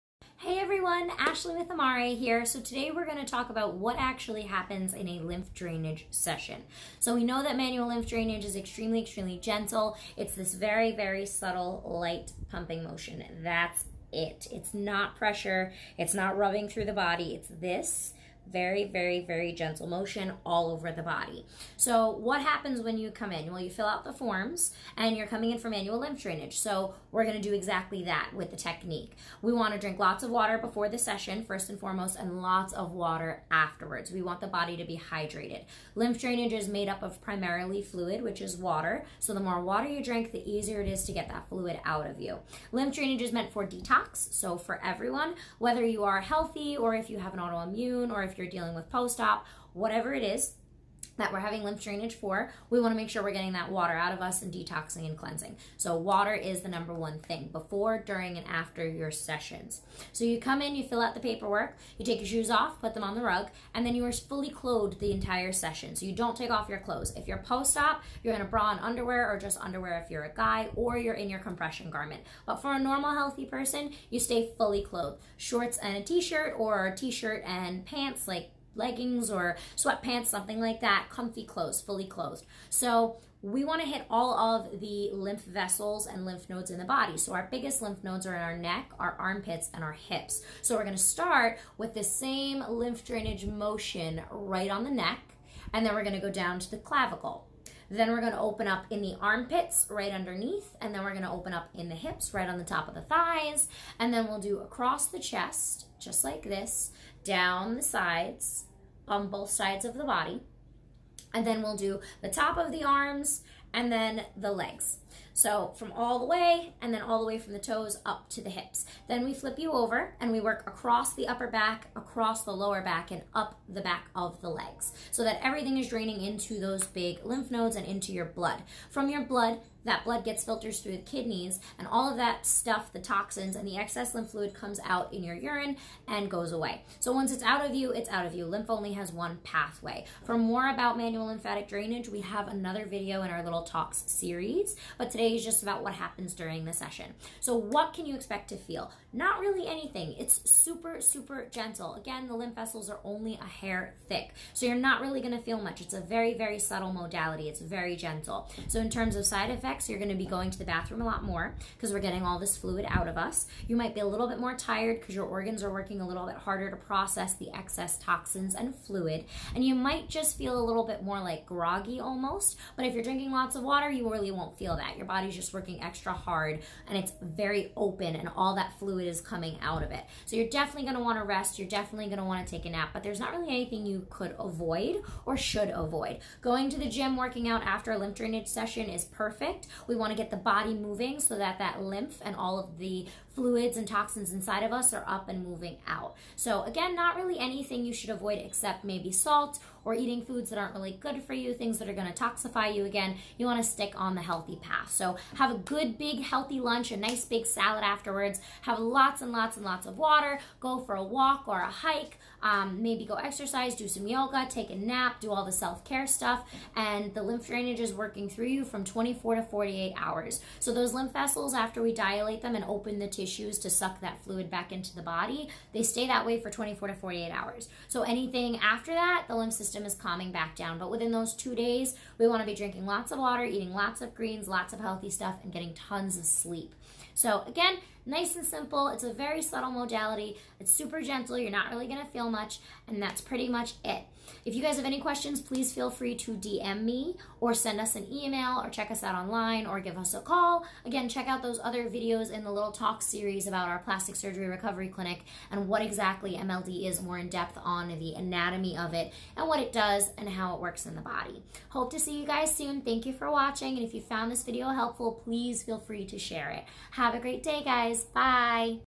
What to expect in a Manual Lymphatic Drainage Massage Session
Click here to enroll in our online course to learn what massages you need post-op and how to find a licensed post-op massage therapist:

Our Plastic Surgery Recovery Specialist, Ashley, discusses what exactly happens during a Manual Lymphatic Drainage Session. We already know what the lymph system is and what Manual Lymphatic Drainage is, so it's important to know what actually happens in a session! Ashley tells you what you need to expect to happen during your typical session.

Locations:
Highland Park and Elizabeth, NJ

0:00 - 0:59 - Important to know before you session!
1:00 - 2:26 - What to wear and what happens
2:28 - 4:29 - What can you feel?